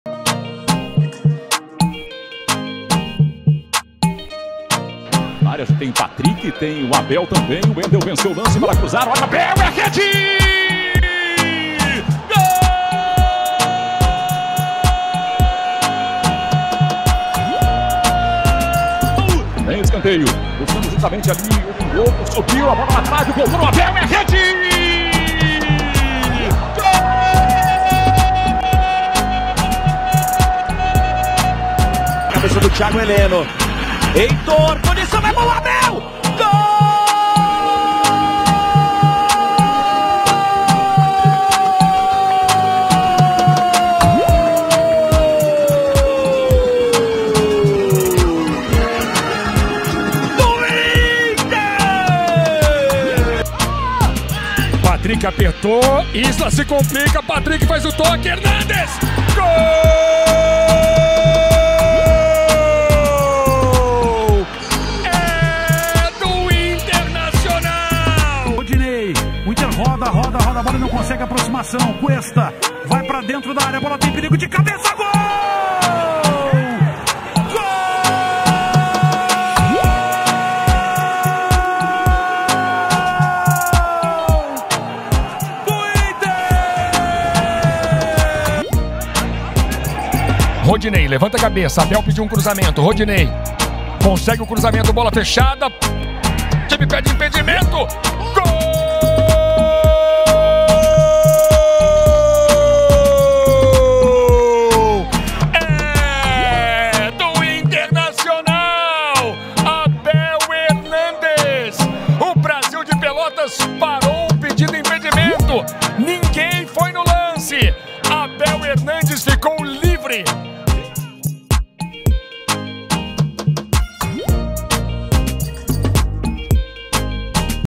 TODOS os 6 Gols de ABEL HERNÁNDEZ Pelo INTERNACIONAL | Em Ordem Cronológica
Deixe sua sugestão para futuros vídeos nos comentários.

Todos os 6 GOLS de ABEL HERNÁNDEZ pelo Internacional

Muito obrigado pelo apoio e DALE INTER!

Jogos:
16/09/2020 - Internacional 4 x 3 América de Cali-COL - Libertadores 2020
11/10/2020 - Internacional 2 x 1 Athlético-PR - Brasileirão 2020
25/10/2020 - Internacional 2 x 2 Flamengo - Brasileirão 2020
24/01/2021 - Internacional 2 x 1 Grêmio - Brasileirão 2020
27/03/2021 - Brasil de Pelotas 1 x 2 Internacional - Gauchão 2021